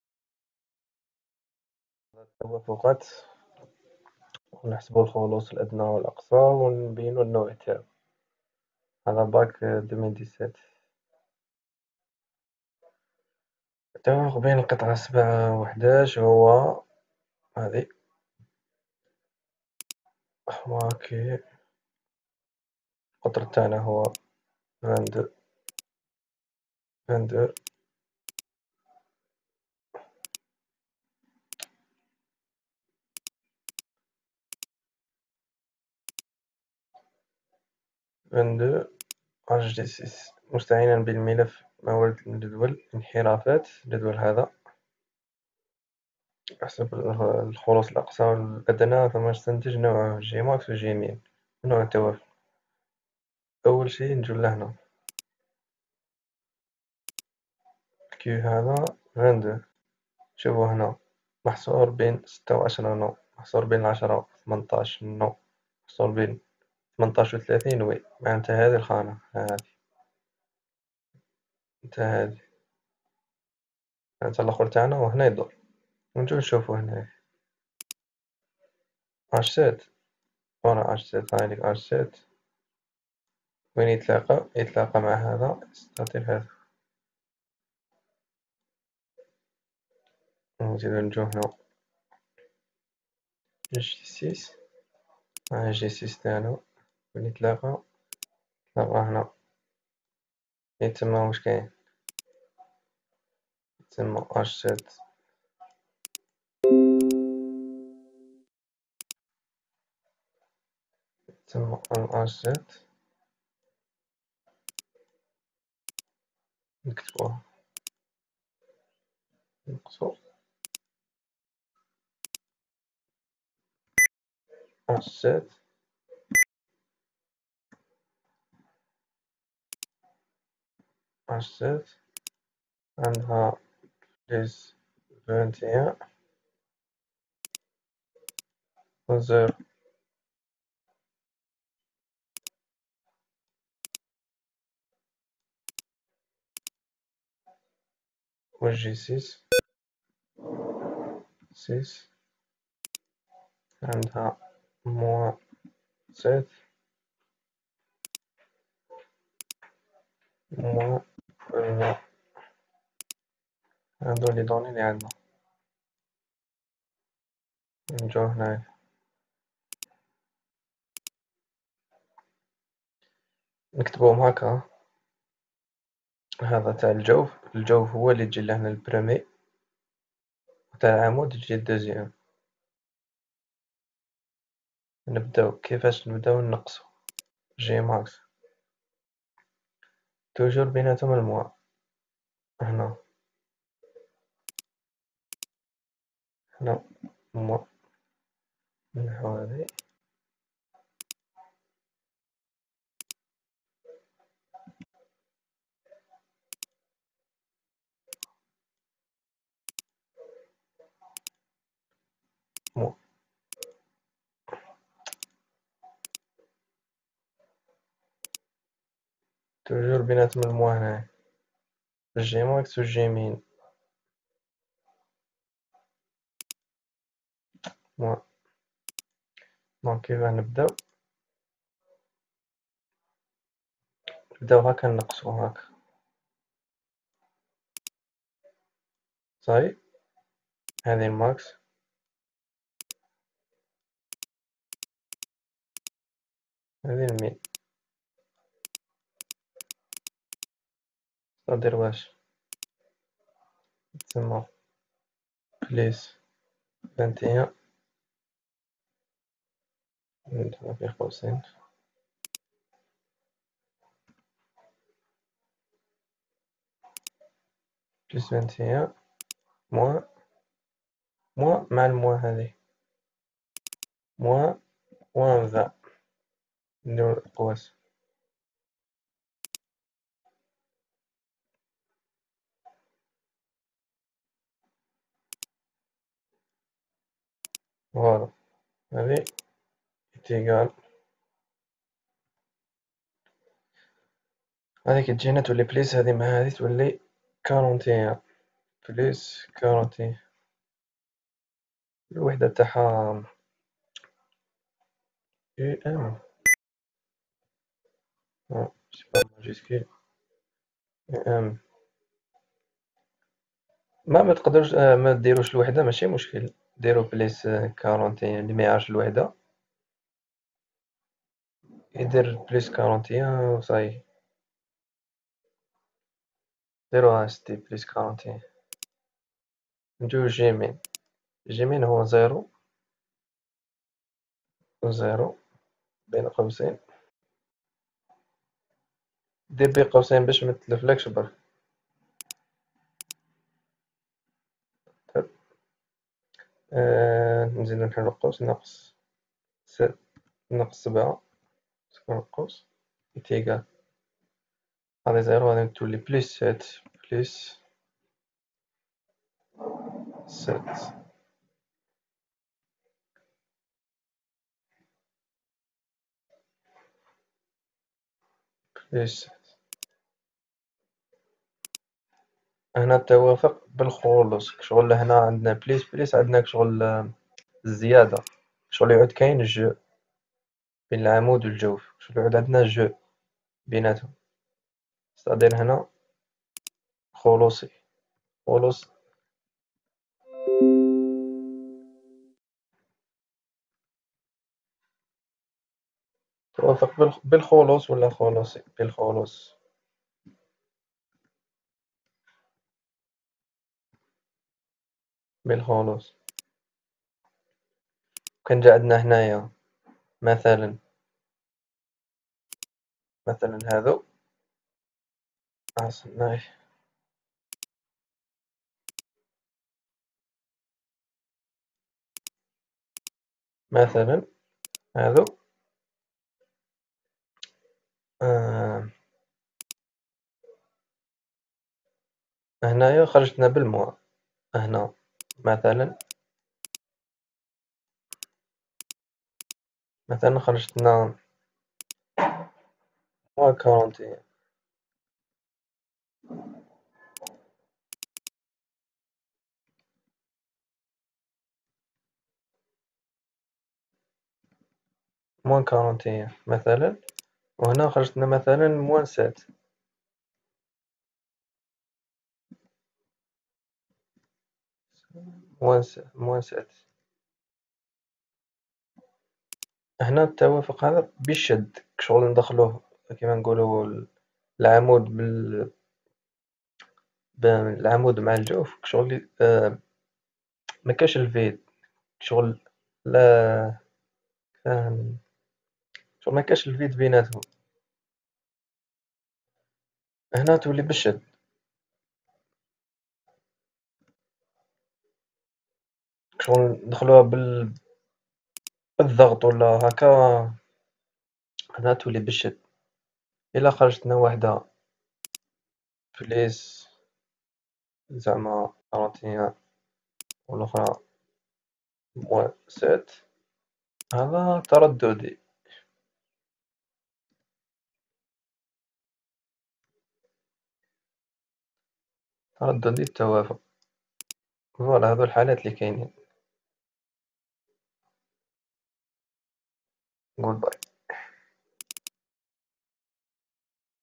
حساب الخلوص الأقصى و الأدنى و نوع التوافق⚙️💕 الهندسة الميكانيكية بكالوريا الأسئلة الأكثر شيوعاً📃
الأسئلة الأكثر شيوعاً في الهندسة الميكانيكية بكالوريا